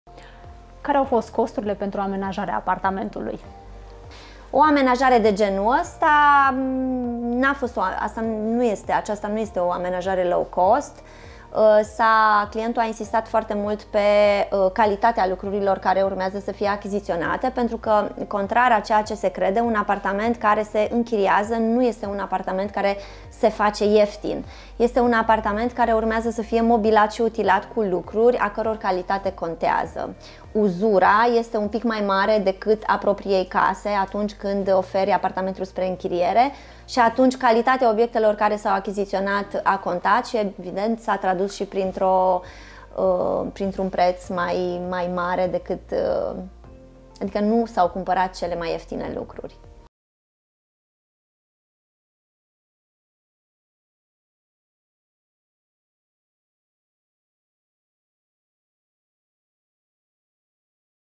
Care sunt costurile pentru amenajarea unui apartament cu 3 camere? | imoExpert #23
O amenajare similară cu cea prezentată în emisiune nu este de tipul low-cost. Clientul a insistat foarte mult pe calitatea lucrurilor care urmează a fi achiziționate.